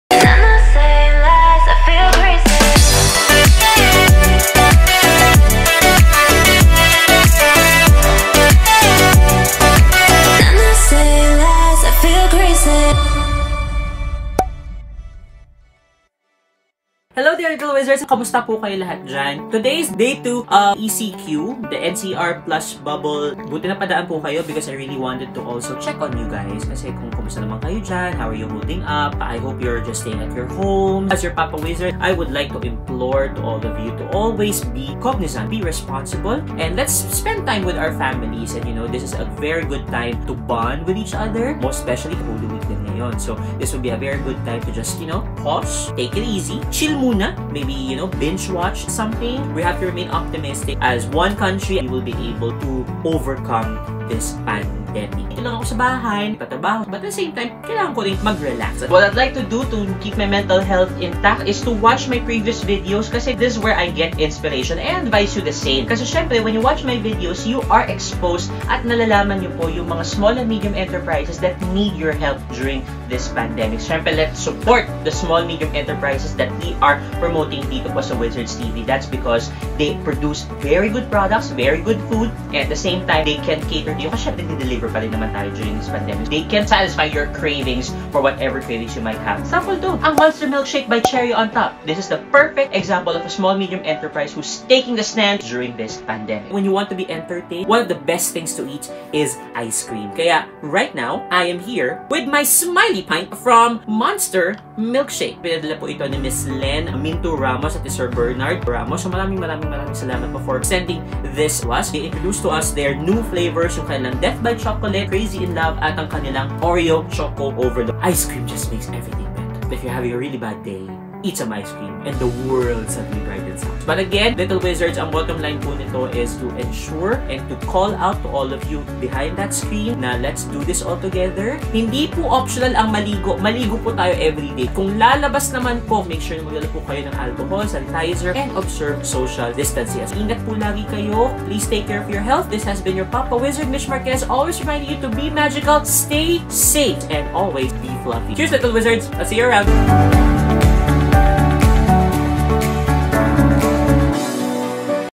KAMUSTA KA, LIL' WIZARDS? | VLOG 055 | WIZARD'S TV
HI LIL' WIZARDS,

I'M AWARE THAT ANOTHER ECQ HAS HIT THE METRO AND POSSIBLE MORE STRINGENT MEASURES WILL BE IMPLEMENTED IN THE COMING WEEKS DUE TO SIGNIFICANT INCREASE IN COVID-19 INFECTION. JUST DROPPING BY TO SAY, KAMUSTA KA? I JUST PRAY THAT YOU AND YOUR ENTIRE FAMILY IS COPING WELL AMID PANDEMIC. HOPING THAT YOU HAVE FOOD ON YOUR TABLE AND EVERYONE IS HEALTHIER THAN EVER. PLEASE STAY SAFE AT ALL TIMES! IN TIME, WE'LL DEFINITELY GET THRU THIS. I KNOW THAT THIS MAY SOUND CLICHE BUT PLEASE STAY OPTIMISTIC. THAT'S THE BEST HELP YOU CAN GIVE TO YOURSELF IN THIS TRYING TIMES. I LOVE Y'ALL MY LIL' WIZARDS! -XOPAPAWIZARDMITCH